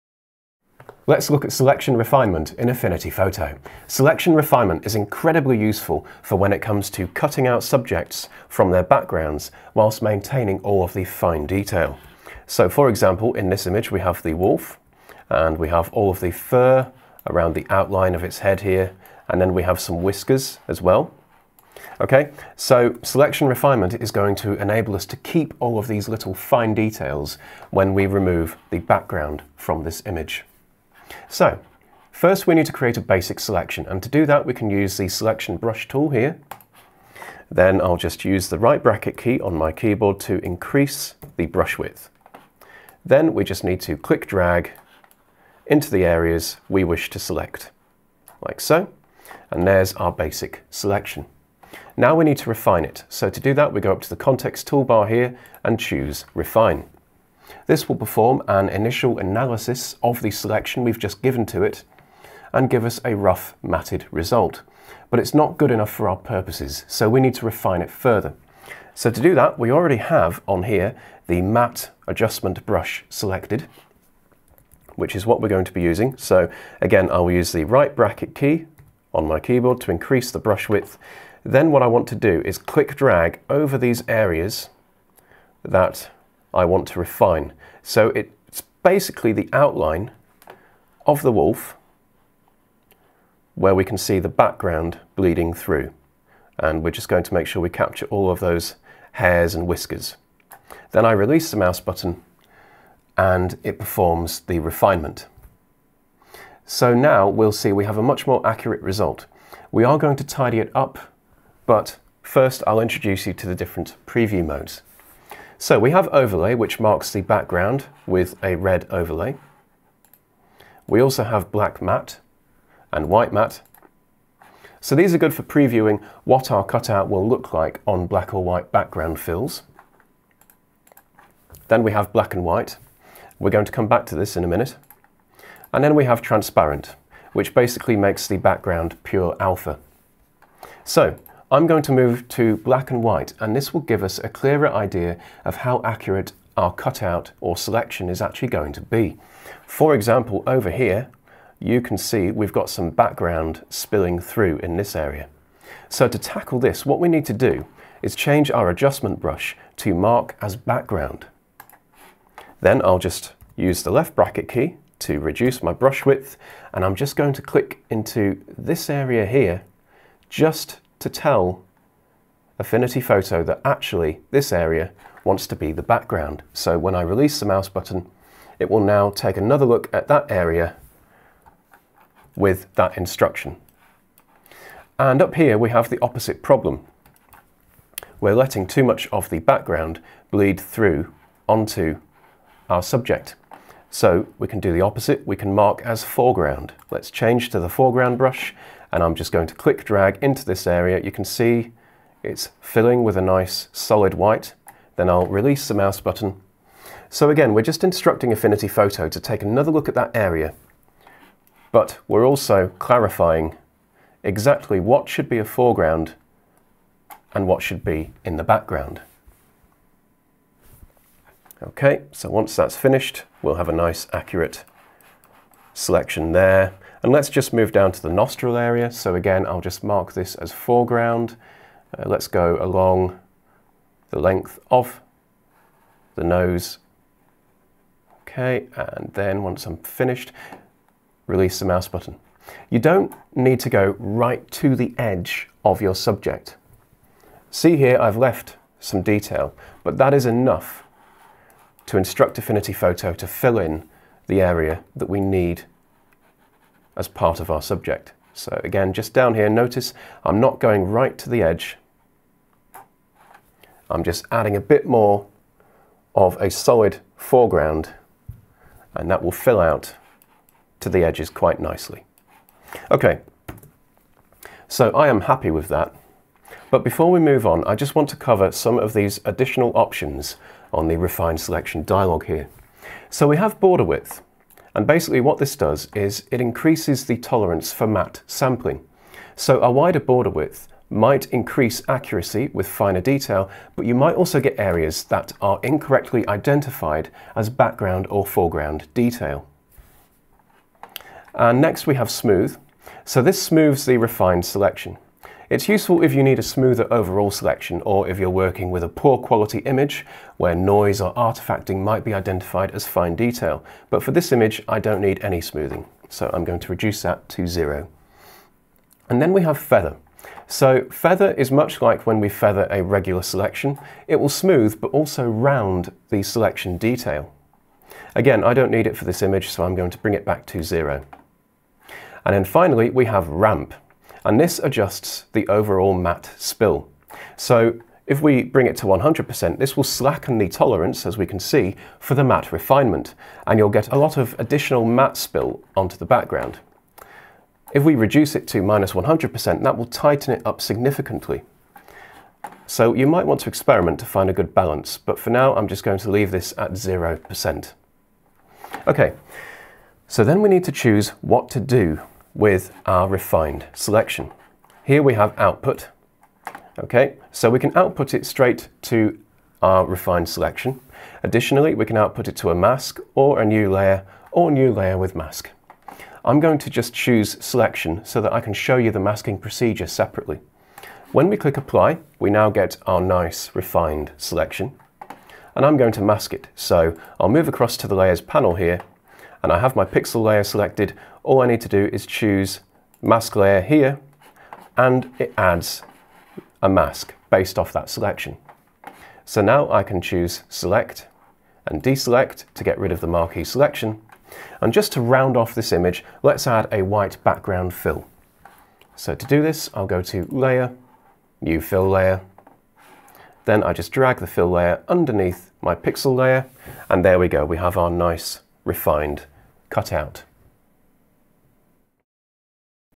Selection Refinement (Affinity Photo)
Refine your selections to create accurate mattes for tricky subjects like hair and fur.

Photography by James Ritson.